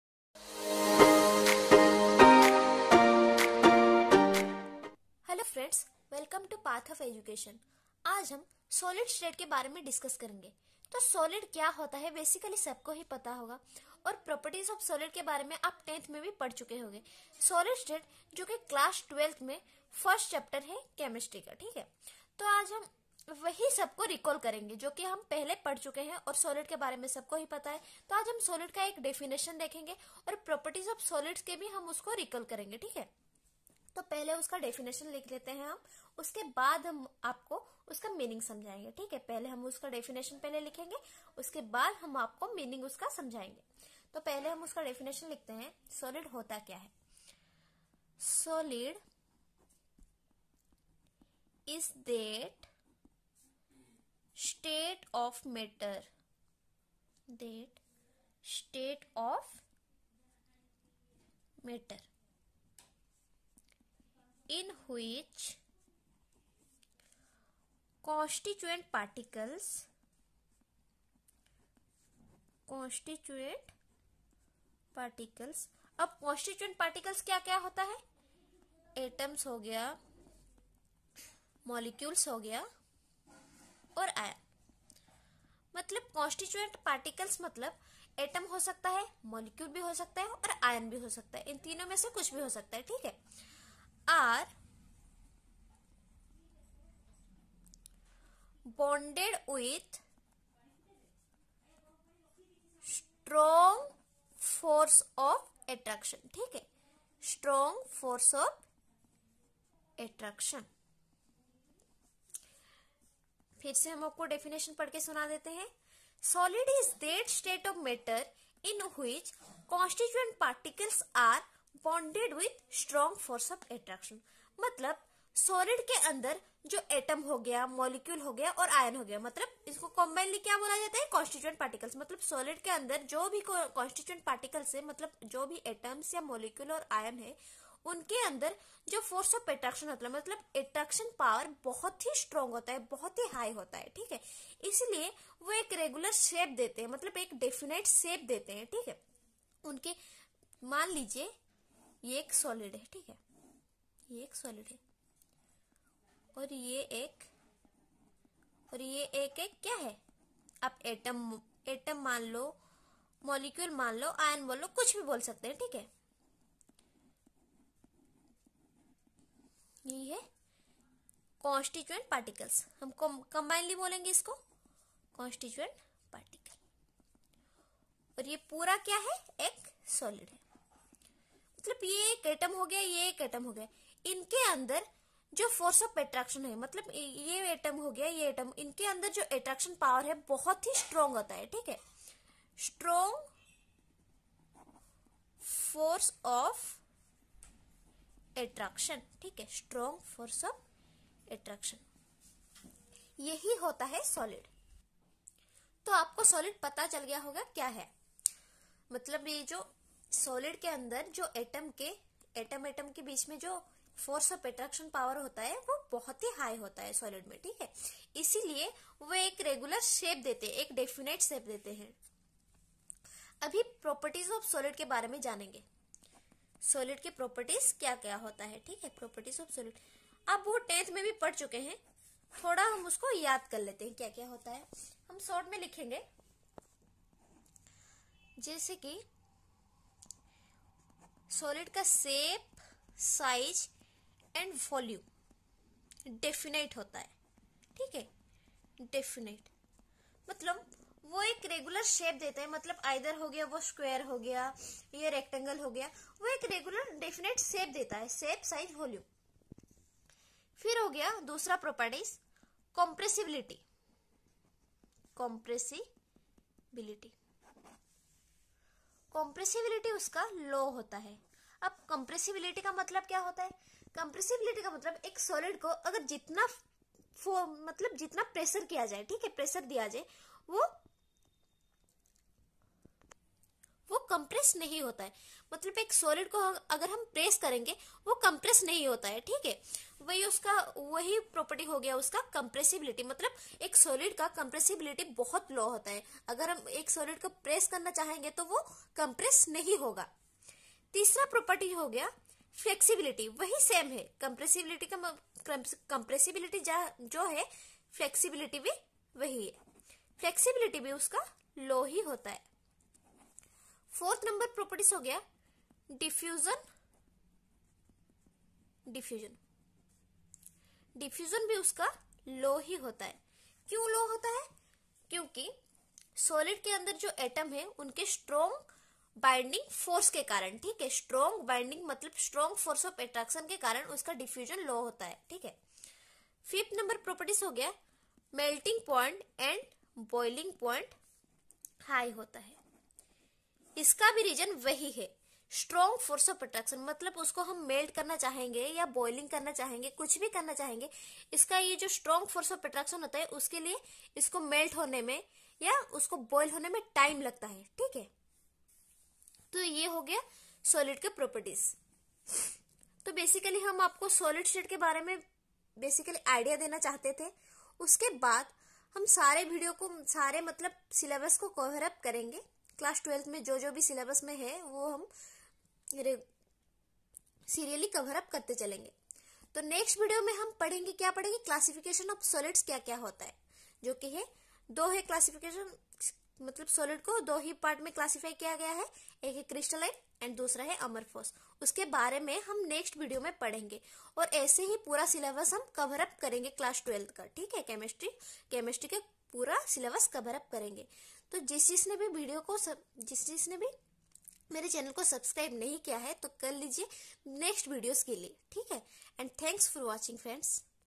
The Solid State || What is Solid? || Properties of Solid? || Class 12th Chemistry
The solid state
Teach about solid state and it's matter.